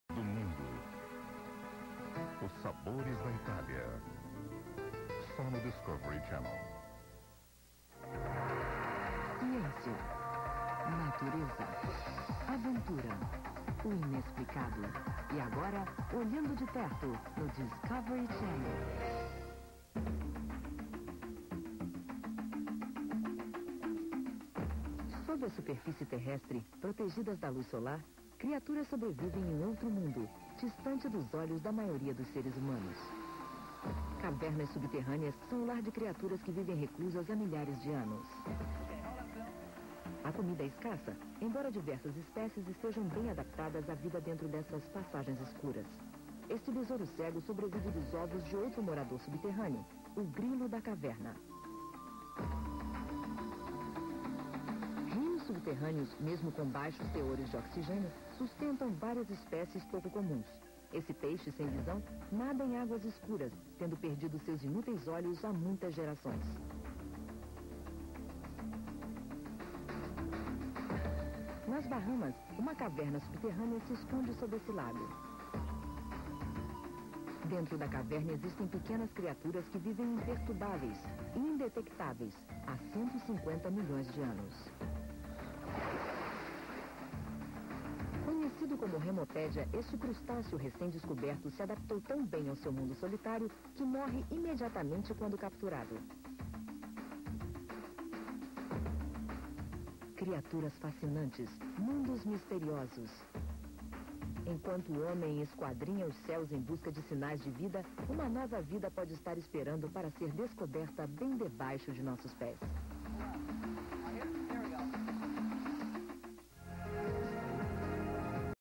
Intervalo do Canal Discovery inicio do ano 2000
Esse intervalo foi tirado da fica k7 no inicio do ano 2000 o DVD estava nascendo e existiam muitos vídeos k7 da uma olhada e mate a saudade